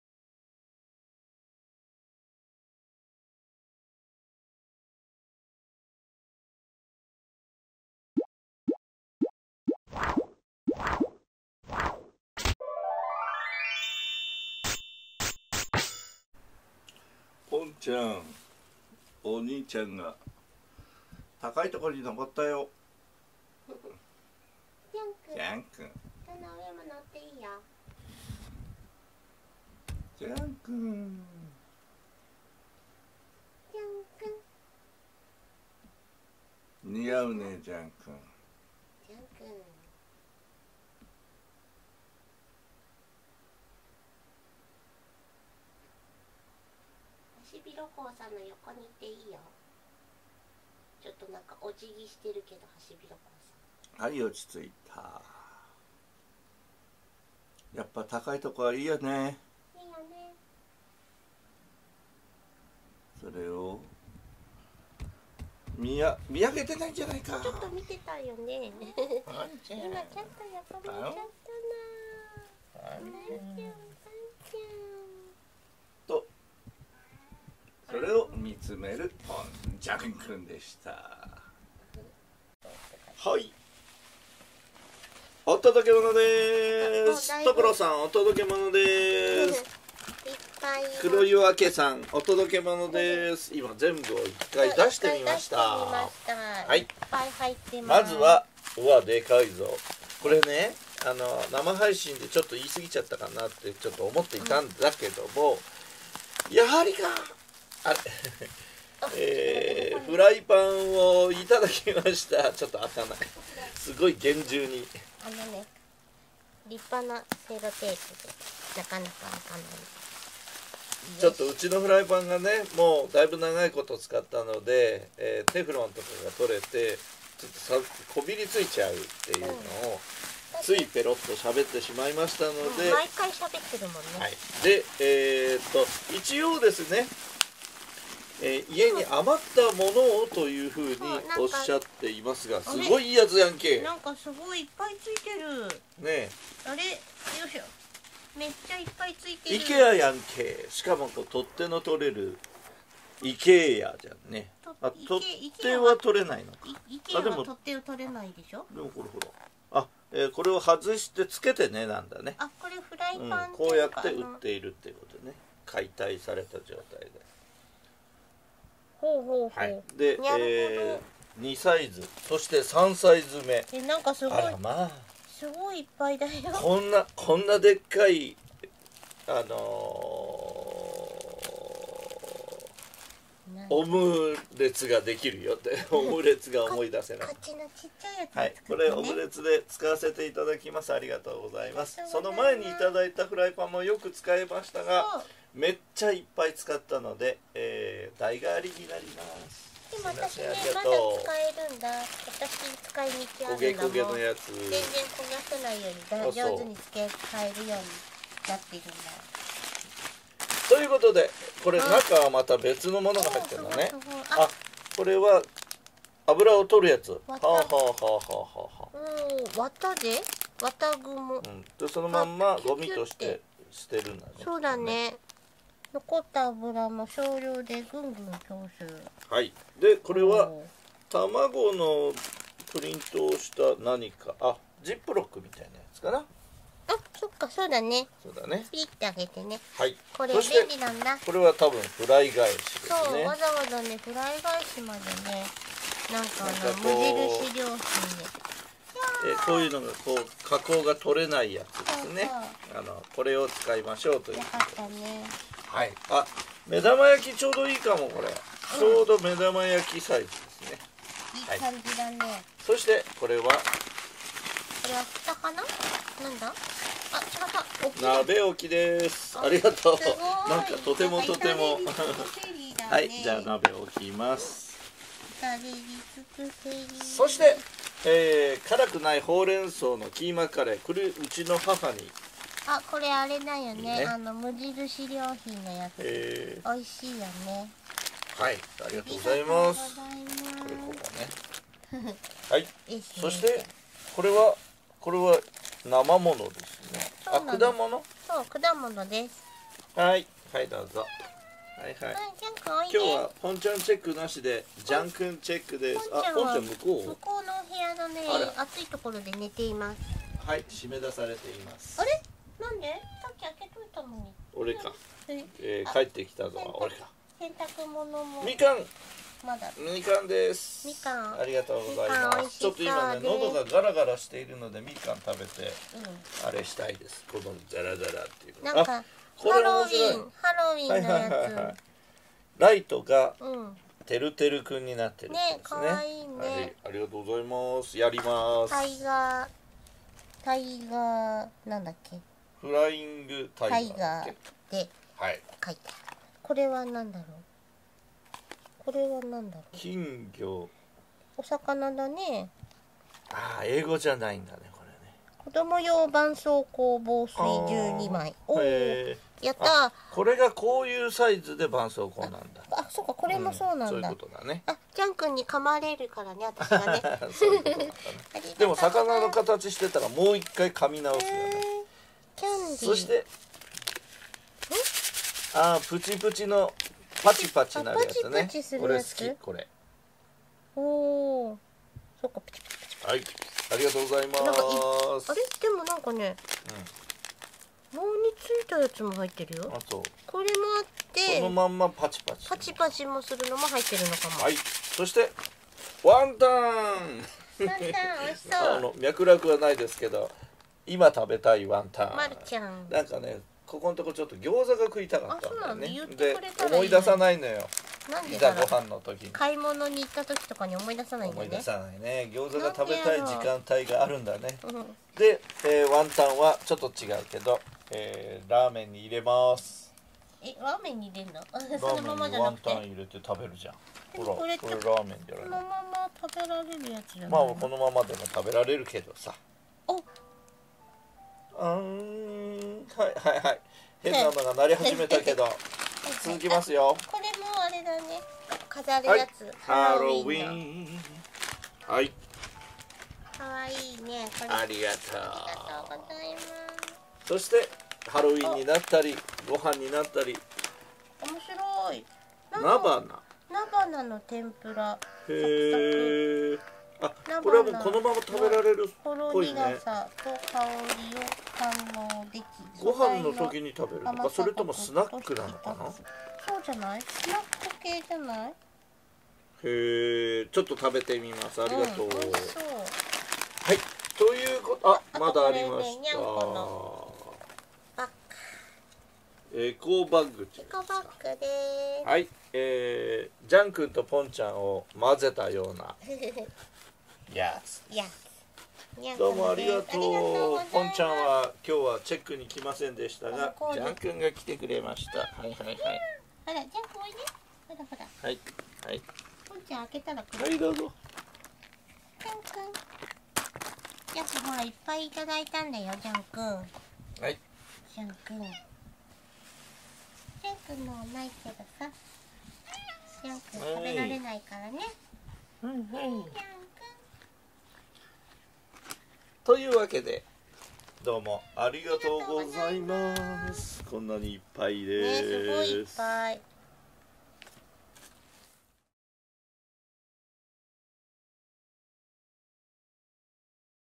生活用品をいただきました！美味しいオムレツ作ります【Jean & Pont 3333】2023/9/16 保護猫ジャンけんポン
とてもありがたいです。
朝ごはんの担当をパパが受け持つことが割とあるのですが、目玉焼きとオムレツの焼き方に変にこだわりを発揮していい感じにできると上機嫌です。
以前やはりいただいたオムレツようのこびりつかないフライパンがとても気に入って毎日毎日使っていましたが、流石に経年劣化はしてきてその効果が切れたのを生配信で話してしまったからなんでしょう。
いいフライパンが届きました。
もう一回言います、ありがとう！